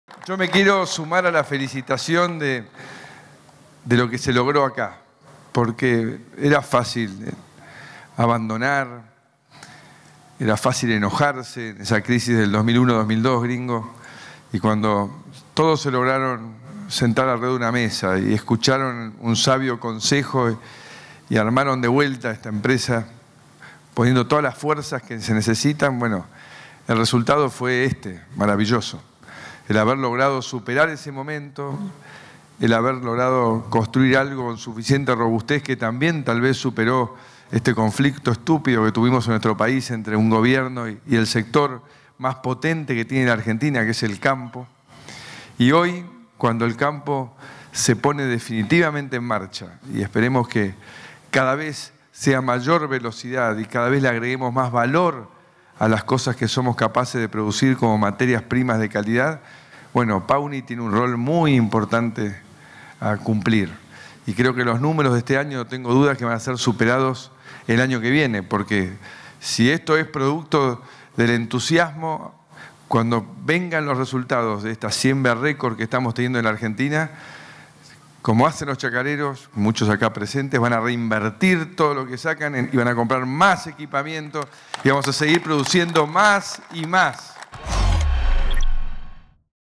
Macri visitó una empresa productora de maquinaria agrícola en Córdoba
El presidente Mauricio Macri entregó el tractor número 15 mil de la empresa de maquinaria agrícola cordobesa Pauny, acompañado por el gobernador provincial Juan Schiaretti y el intente de Las Varillas, Daniel Chiocarello.